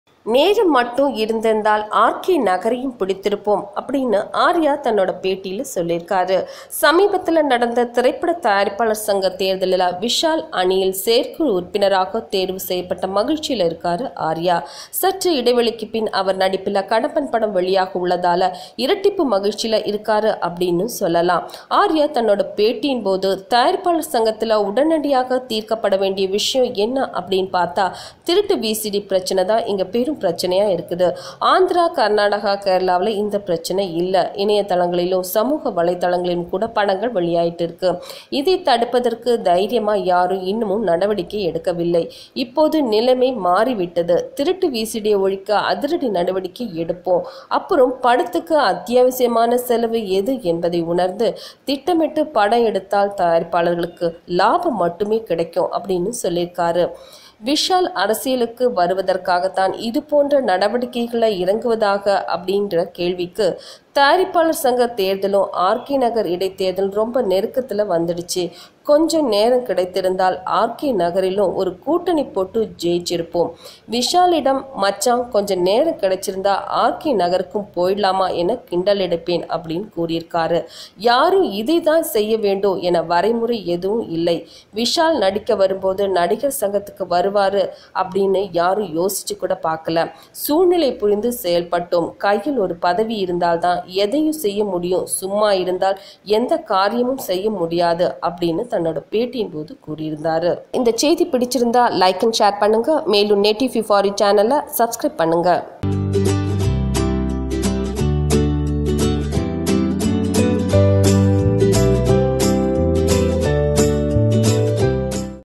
Vishal Team Will Win In R.K Nagar Election | Shocking Statement By Arya | Producer Council Election
Vishal and Arya are close pals. In the Producer Council, Vishal is the President and Arya is an executive committee member.

“When people rumored that Vishal plans for politics, I funnily said come on buddy, let us have an alliance and win RK Nagar election. None guessed that Vishal will become a Nadigar Sangam leader or Producers’ Council President when he entered the film industry. If we want to do good to the people, a designation is much important,” says Arya.

Arya stated this at the Press Meet of Kadamban, his film directed by Raghava and featured by Catherine Tresa. The film is produced by RB Choudary.